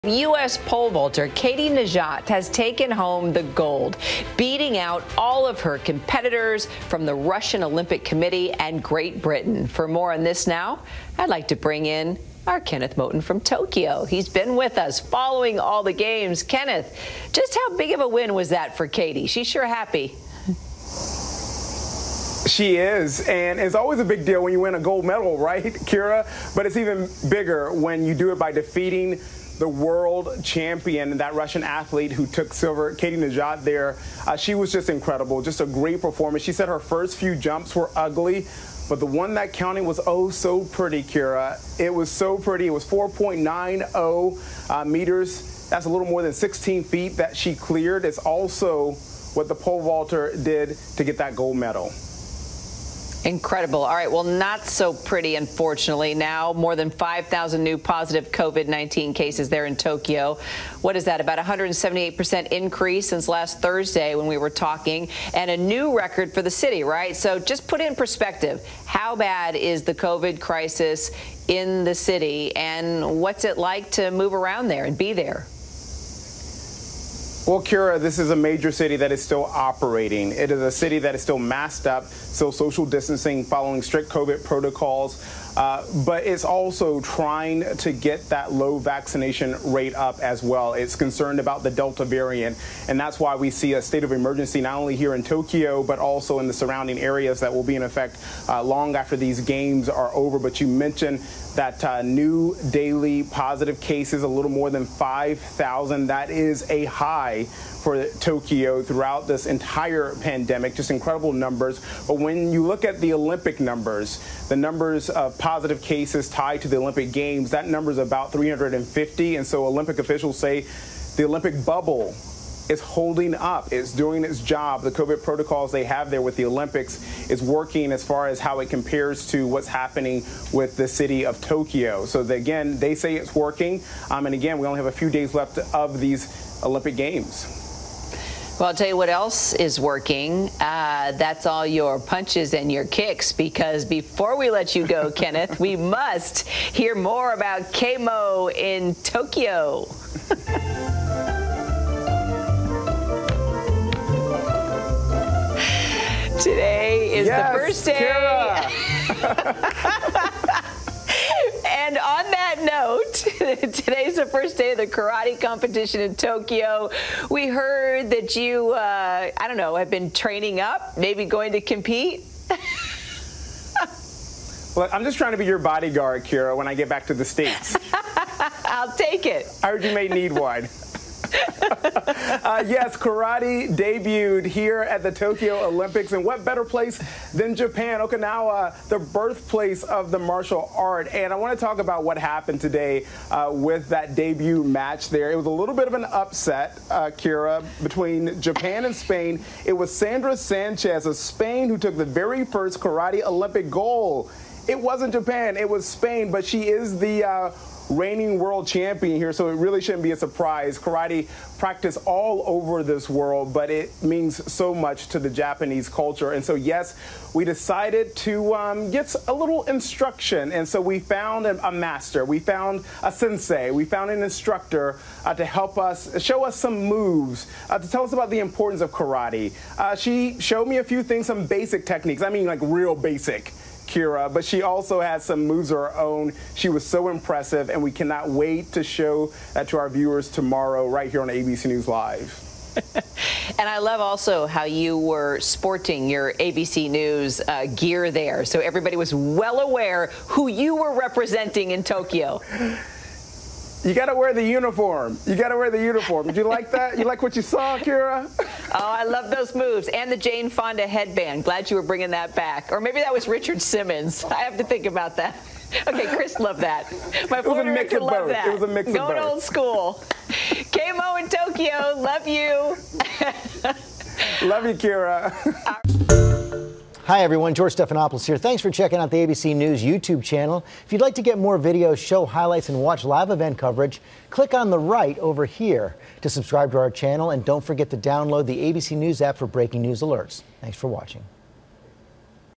US pole vaulter wins gold as COVID-19 cases surge in Tokyo
Katie Nageotte beat out the competition from Russia and Great Britain to bring home the gold.